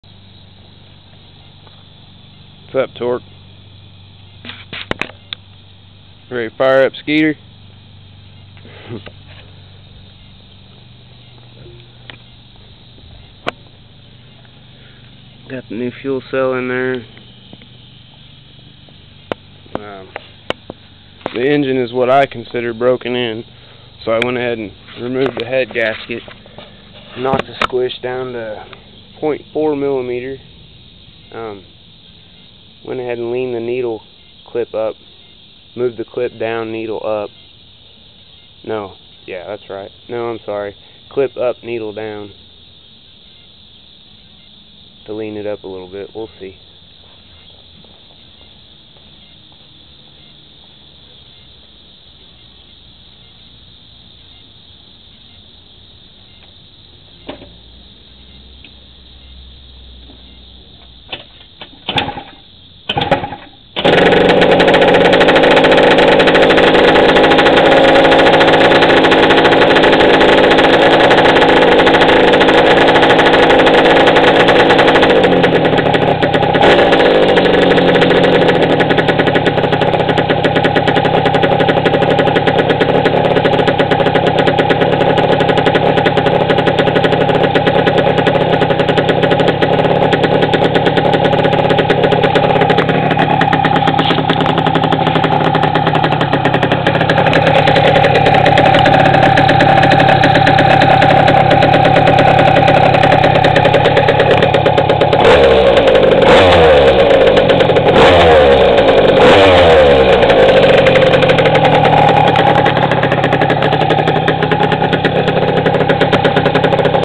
Skeedr 95cc tuning
Not much the vid dosnt say. I ran it up the drive but my SD card on the cam maxed out before i got it. Seems stout!